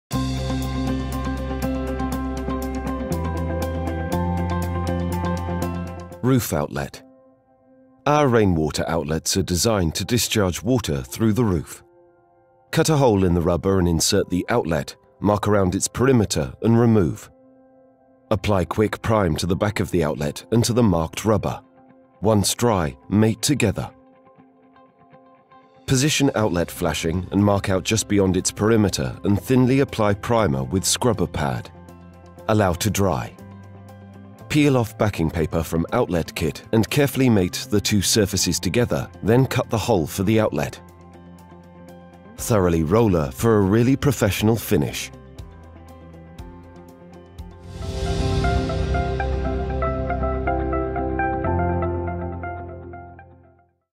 RUBBASEAL | Roof Outlet
How to install roof outlet using Firestone RubberCover Roofing System